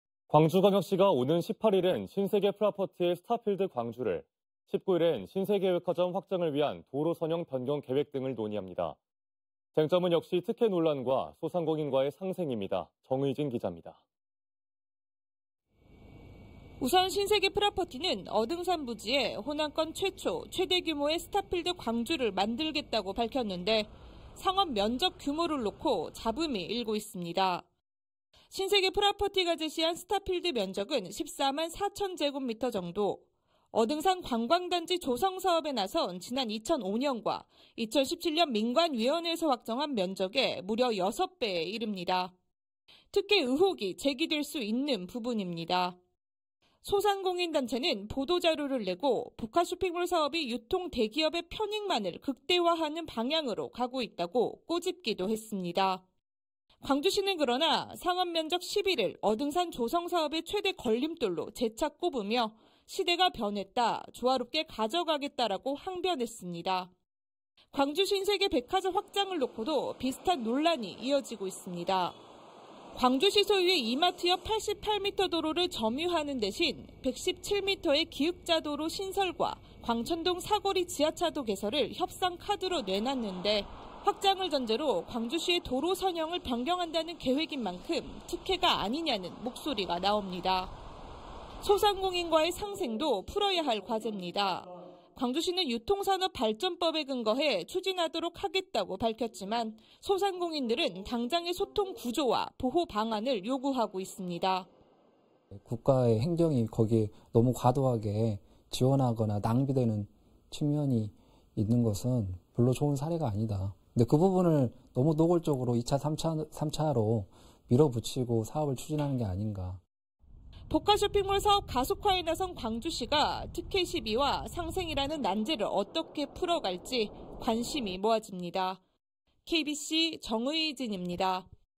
광주 복합쇼핑몰, 결국 쟁점은 특혜·상생 / KBC뉴스
광주광역시가 오는 18일에는 신세계프라퍼티의 스타필드 광주를, 19일에는 신세계백화점 확장을 위한 도로 선형 변경 계획 등을 논의합니다.

쟁점은 역시 특혜 논란과, 소상공인과의 상생입니다.

정의진 기자입니다.